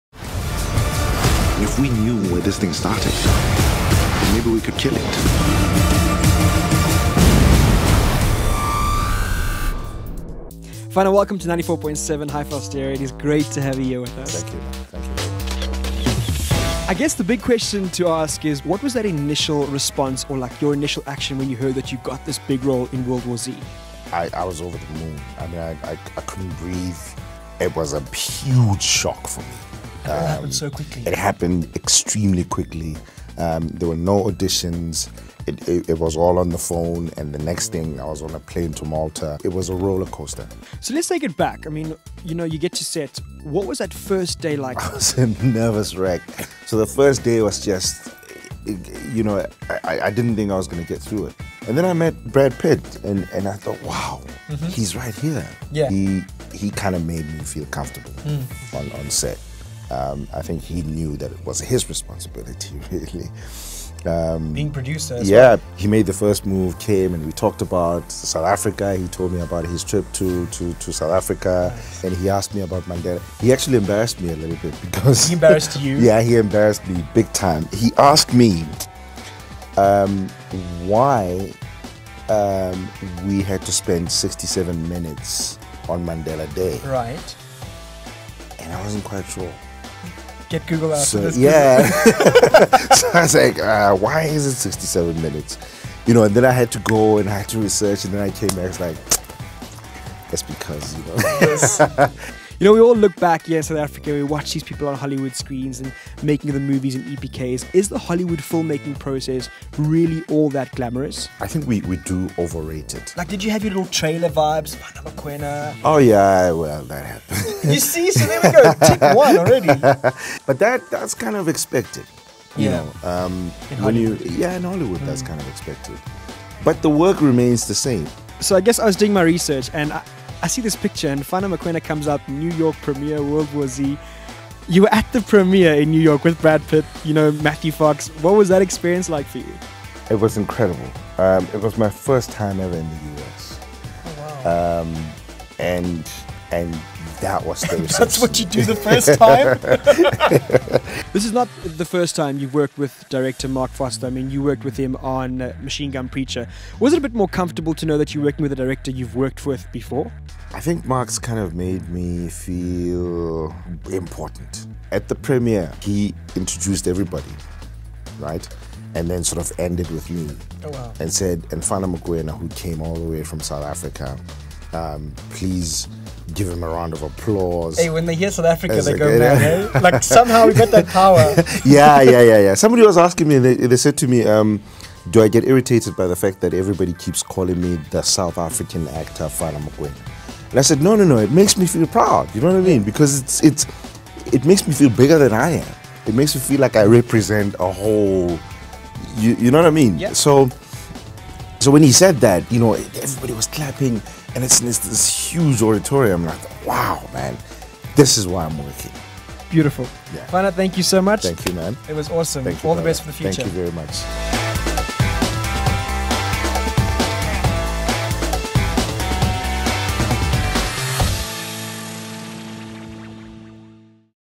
94.7 Interviews Fana Mokoena
Lee-Roy got a one on one interview with Fana Mokoena who co-starred with Brad Pitt in the recent box office hit World War Z. Starts in Cinema's on 19 July 2013